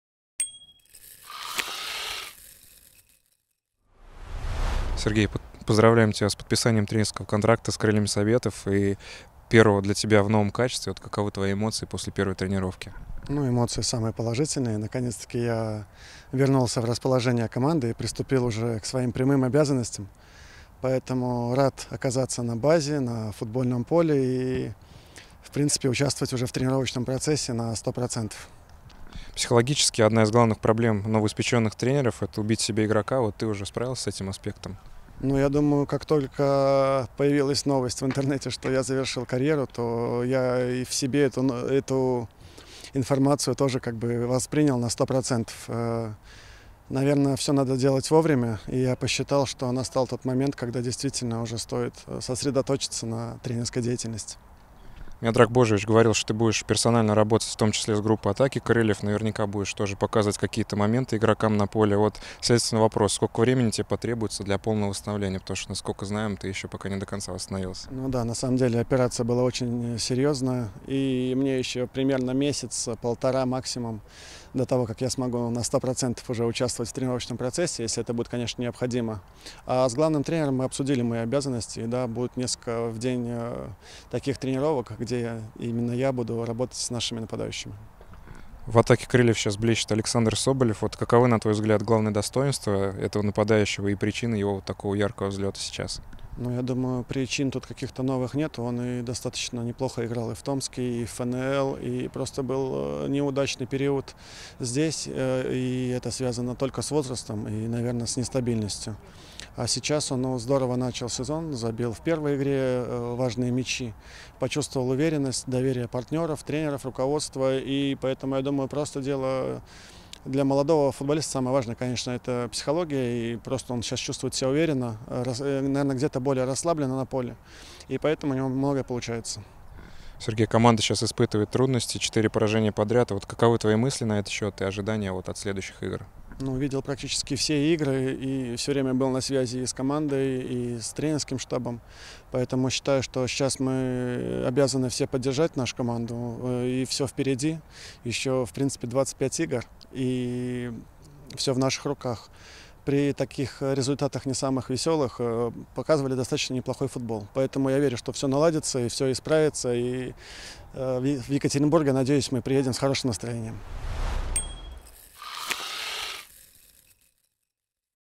Сергей Корниленко: Рад помочь «Крыльям» в любом качестве
Легендарный игрок «Крыльев» Сергей Корниленко завершил карьеру игрока и приступил к тренерской работе. После первой тренировки в новом качестве Сергей ответил на вопросы клубного телевидения.